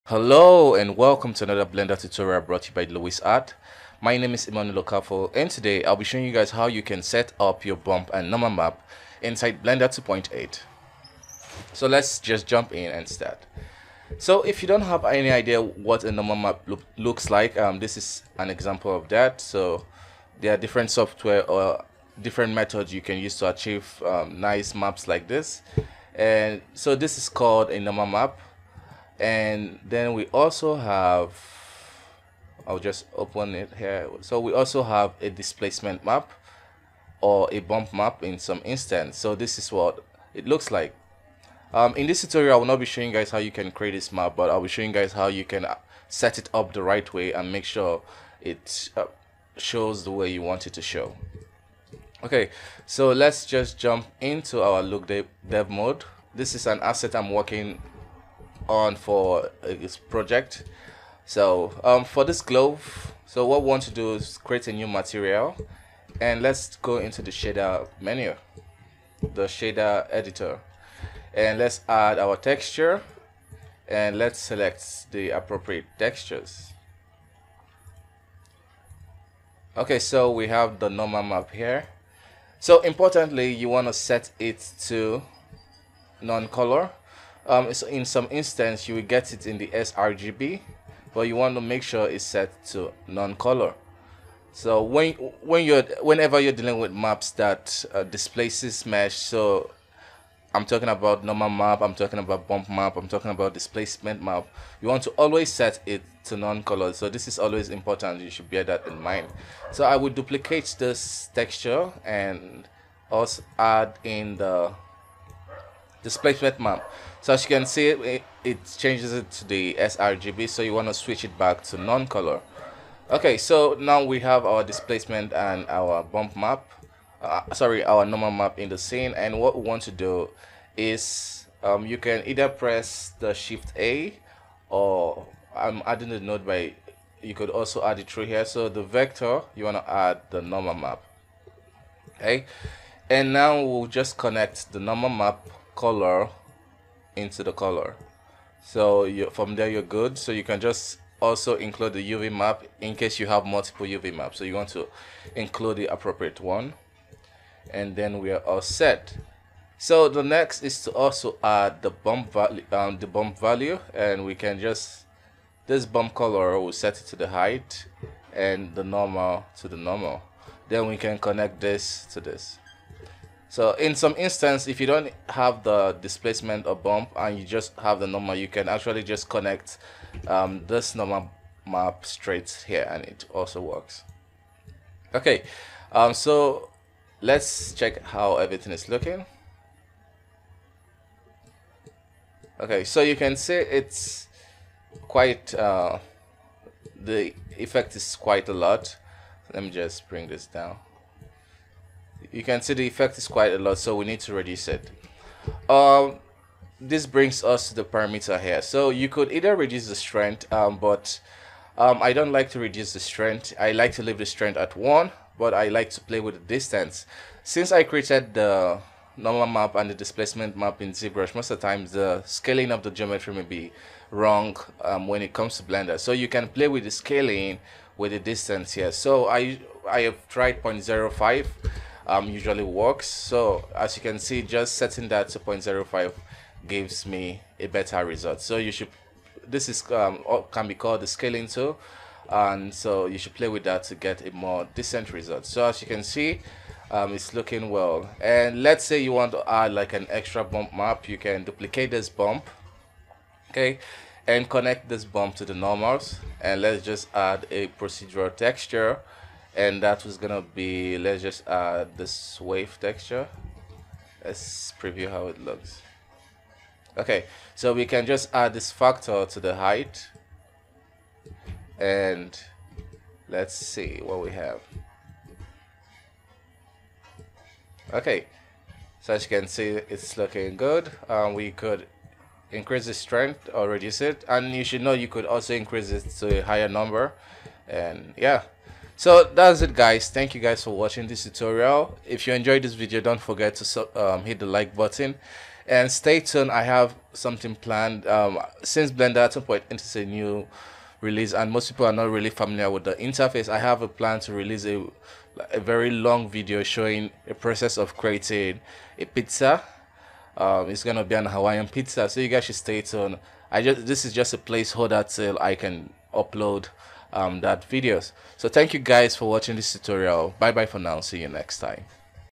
Adding Normal and Bump Maps in Blender 2.80
in this tutorial, i will be showing you how to setup the bump and normal maps the right way in blender 2.80